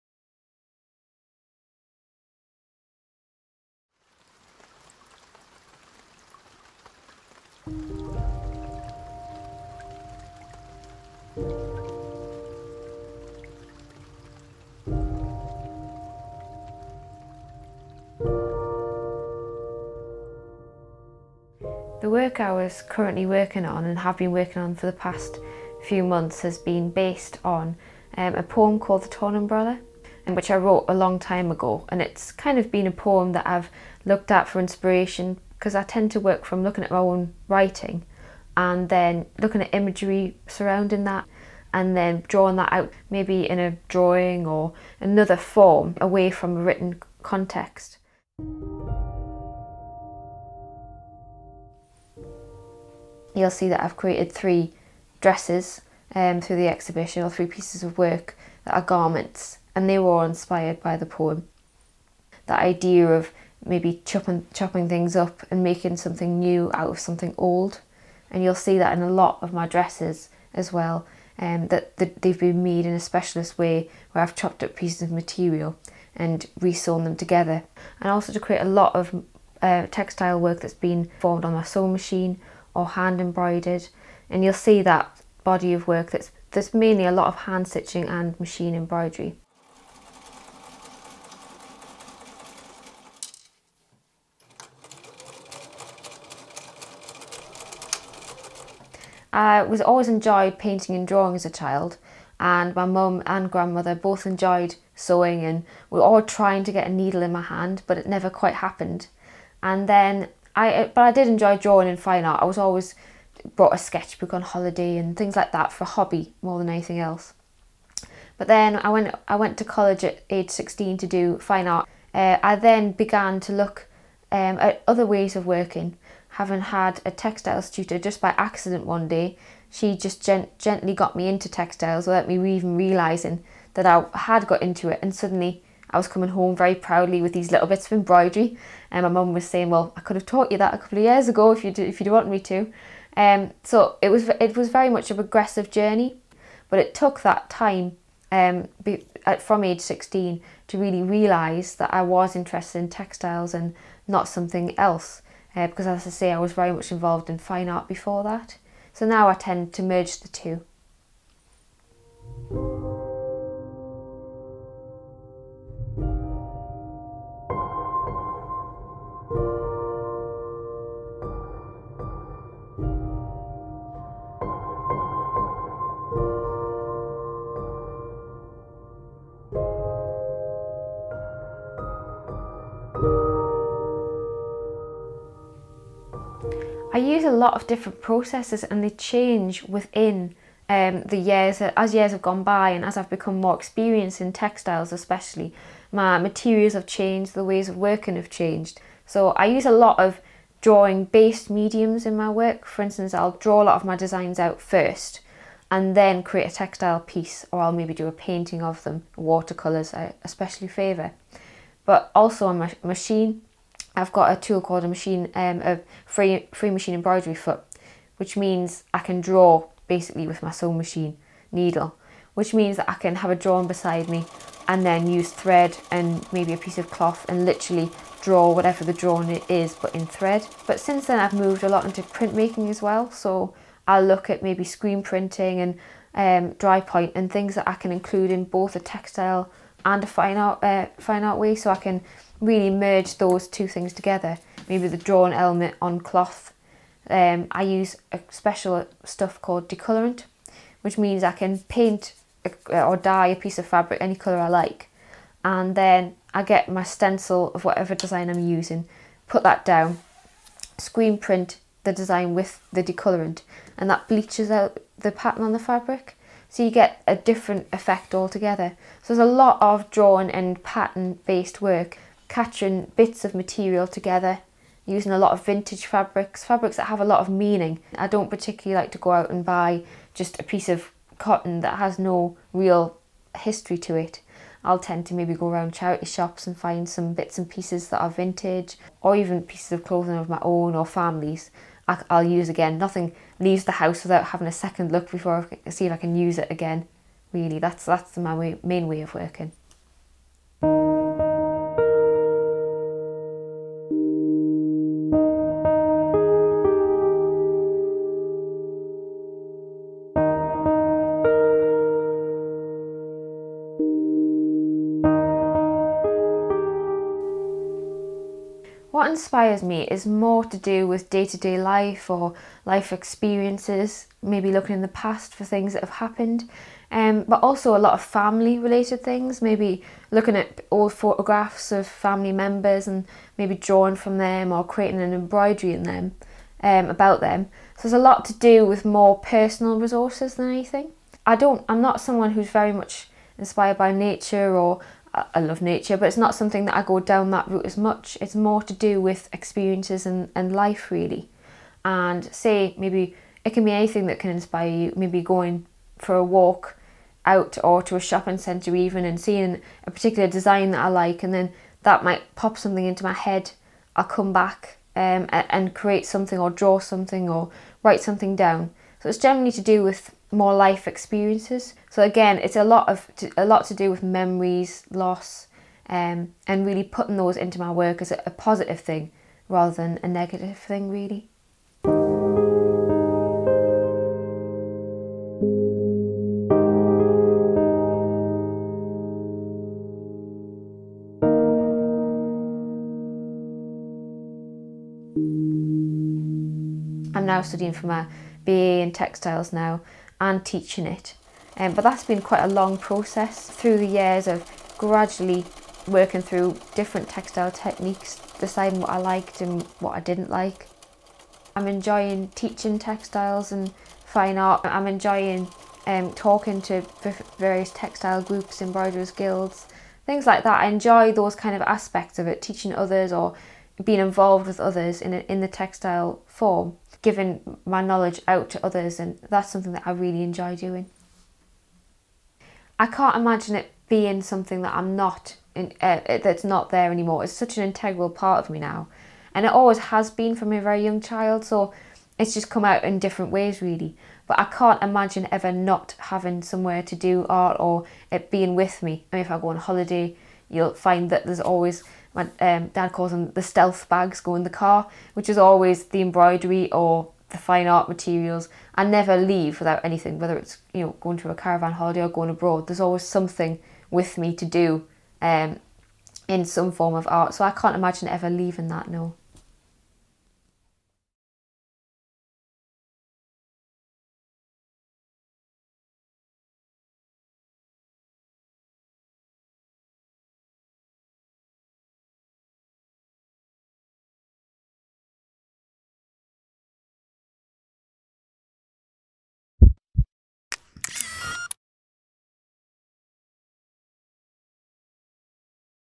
Ailish Henderson - Studio Film 2016
This was filmed recently by We Love Media for Customs House Gallery in South Tyneside, but also as a record of my work.